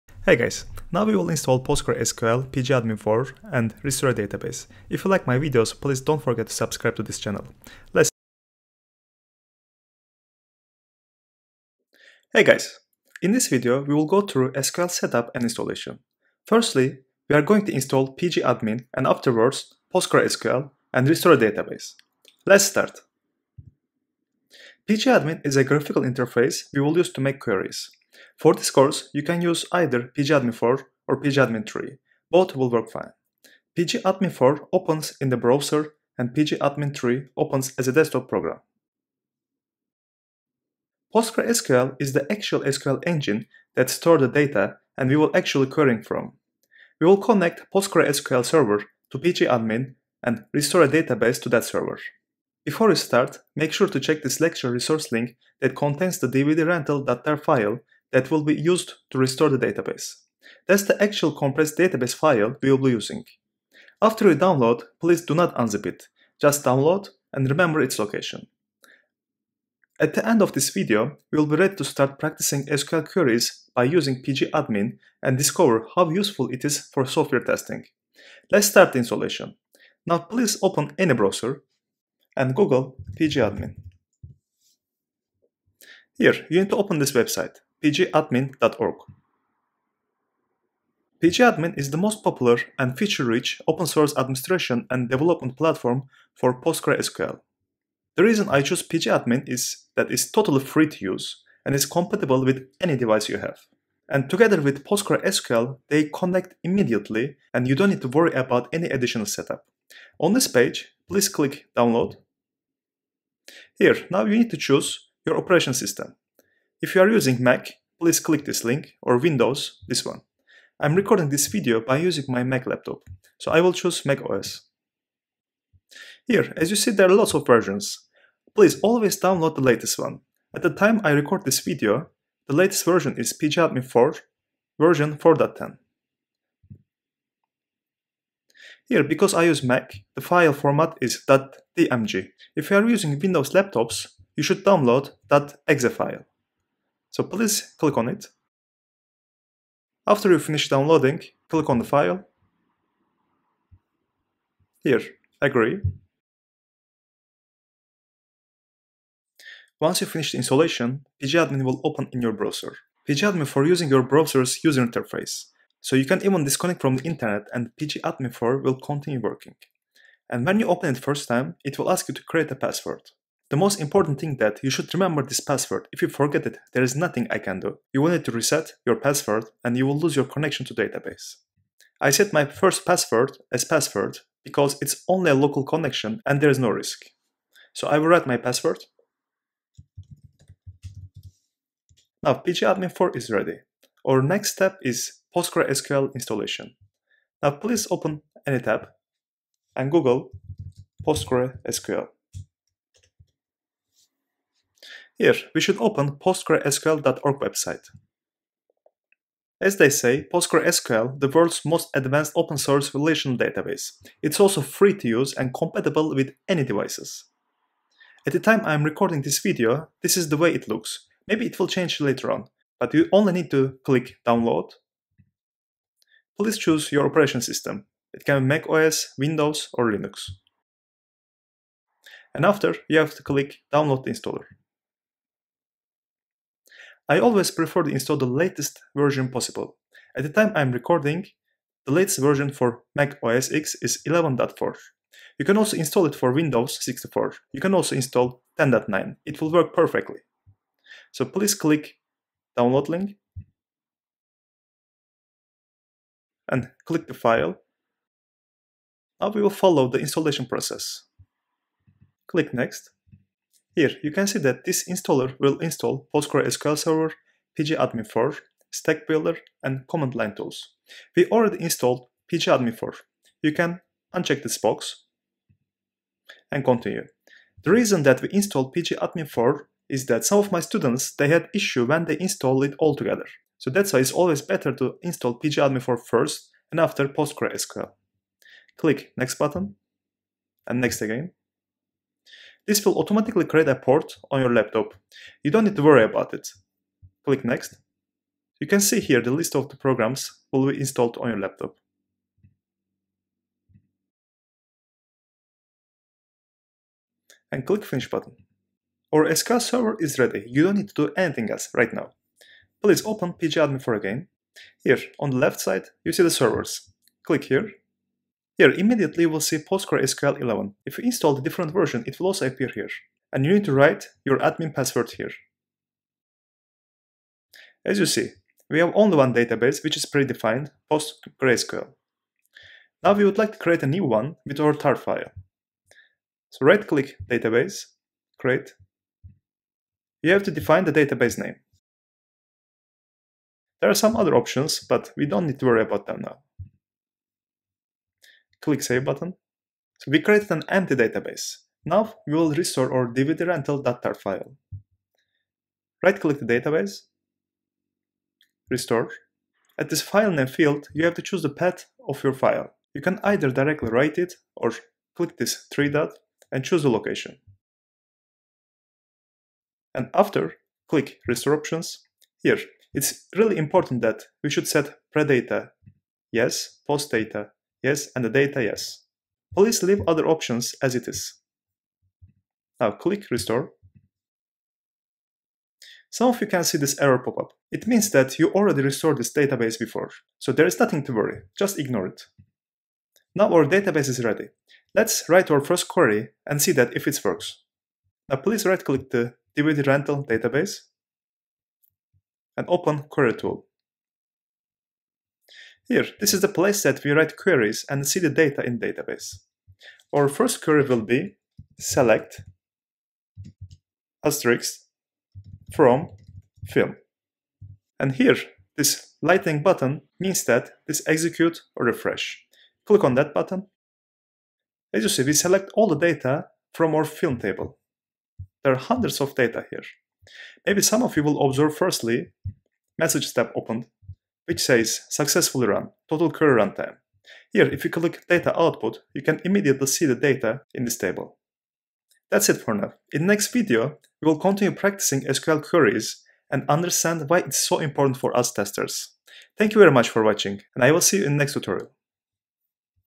SQL Environment and Database setup in 7 minutes! Write your first query
Hey guys! This video is part of my Udemy education. You can purchase my full education on Udemy only for 9,99ct